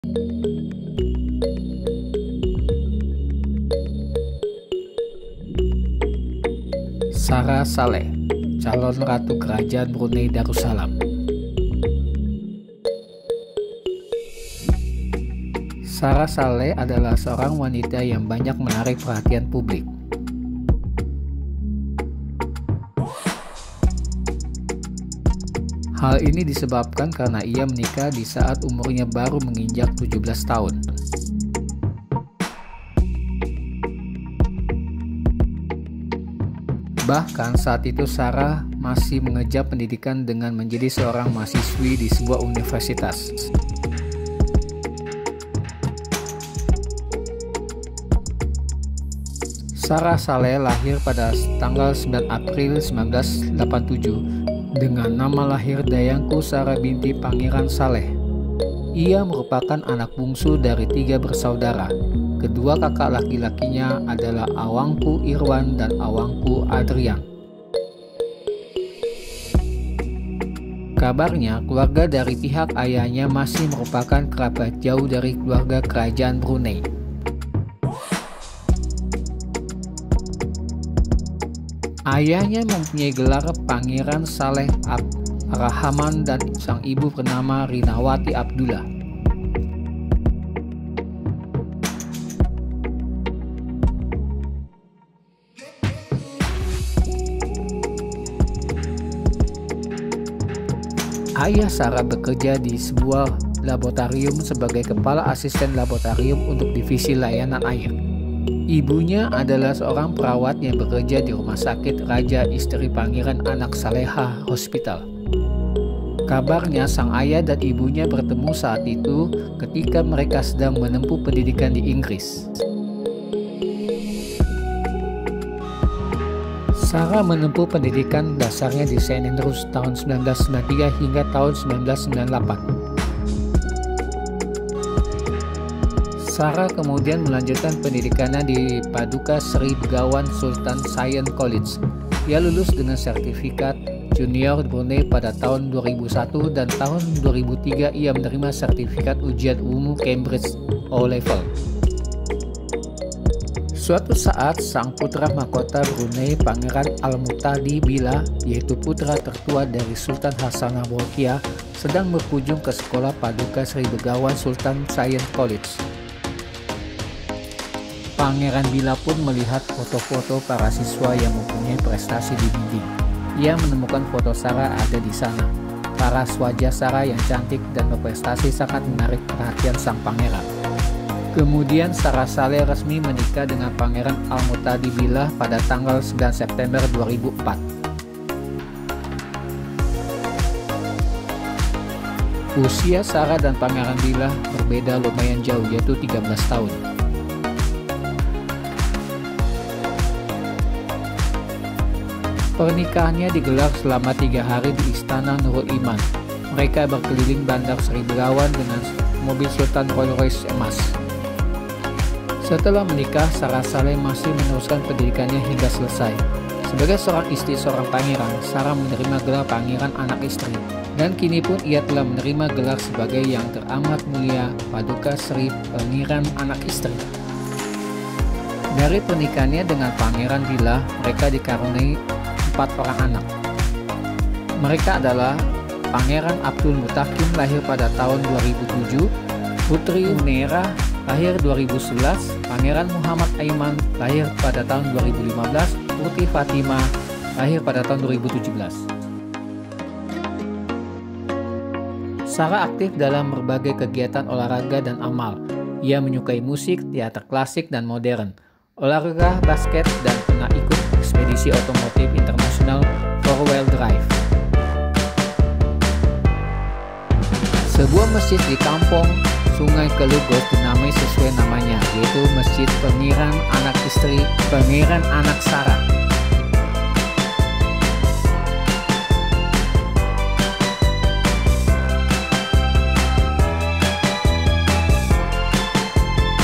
Sarah Salleh, the Beautiful and Achieving Candidate for Queen of Brunei
Sarah Salleh, she is a woman who has many achievements. She is the wife of Prince Billah, Crown Prince of Brunei Darussalam. Since marrying the prince of Brunei, Sarah has also received the status of Crown Princess of Brunei.